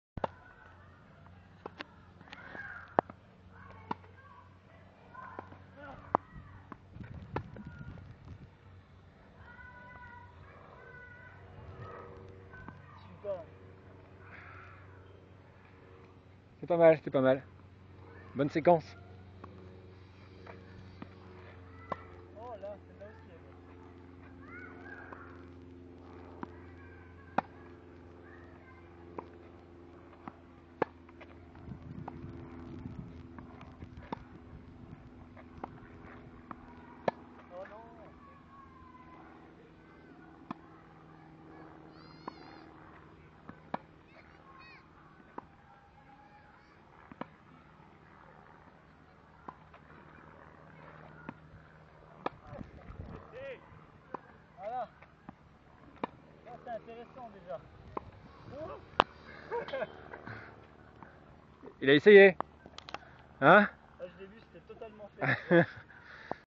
Seb en maillot de bain
Seb donne une leçon de tennis